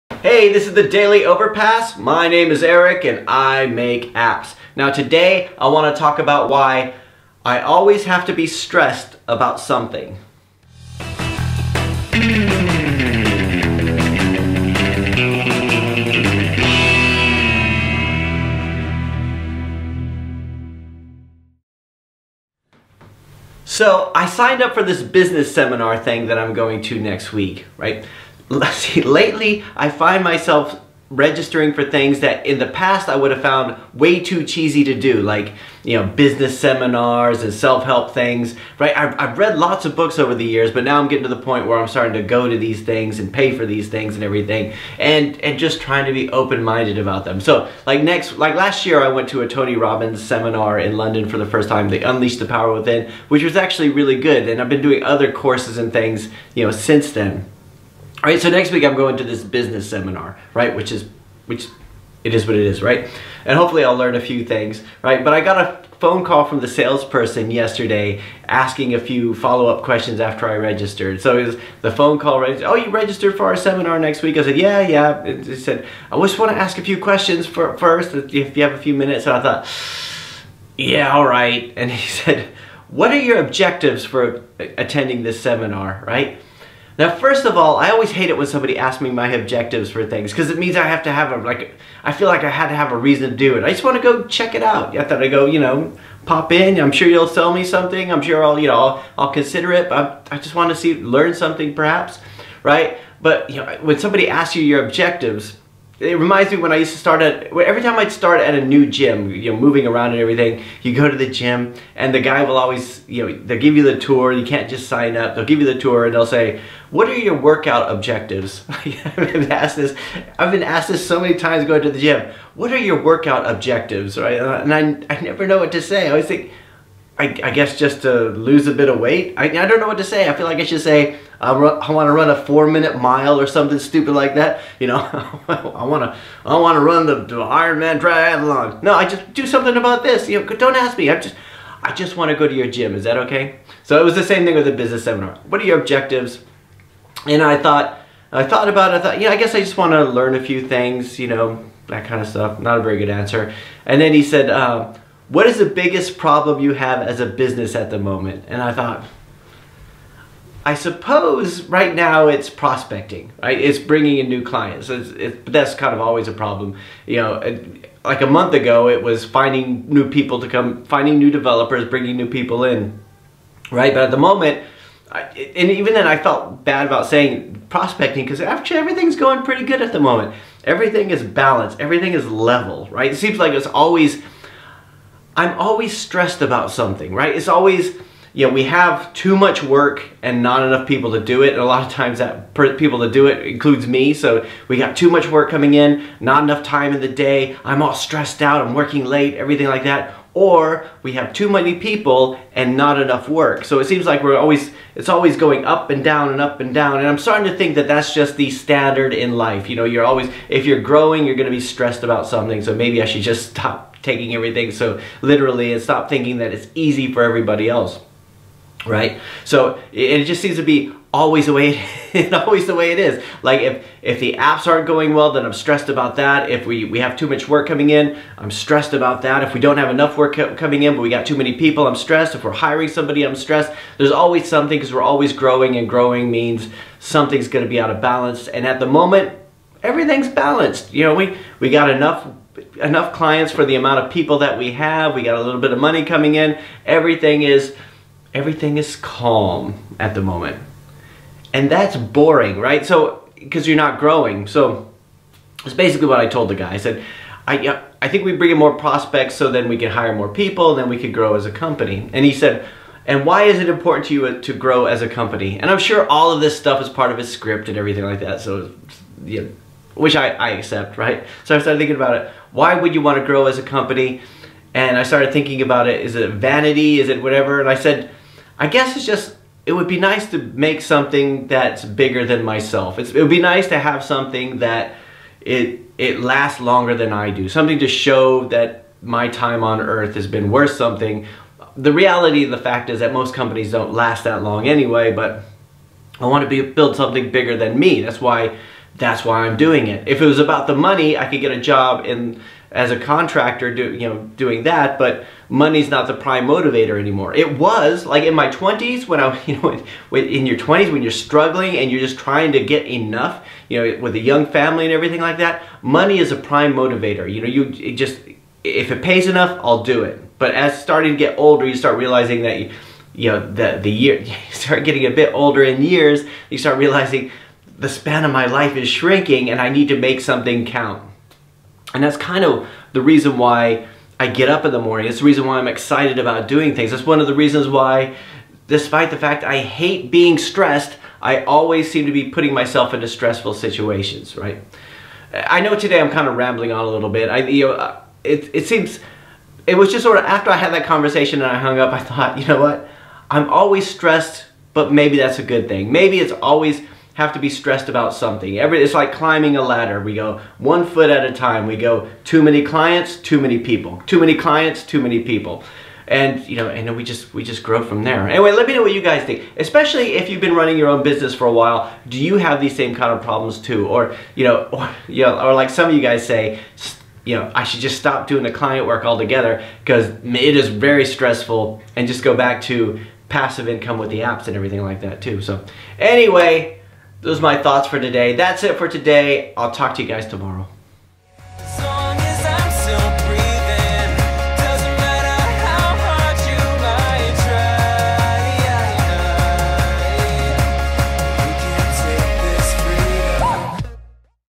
Why I Always Have to be Stressed About Something
Why I Always Have to be Stressed About Something (The Daily Overpass #334)

Today, I talked about why I always have to be stressed about something. I know that it shouldn't be the case but there are things that I can't control and we can't control that would sometimes push me to my limits and would make me make a fuss about.

So...

Or...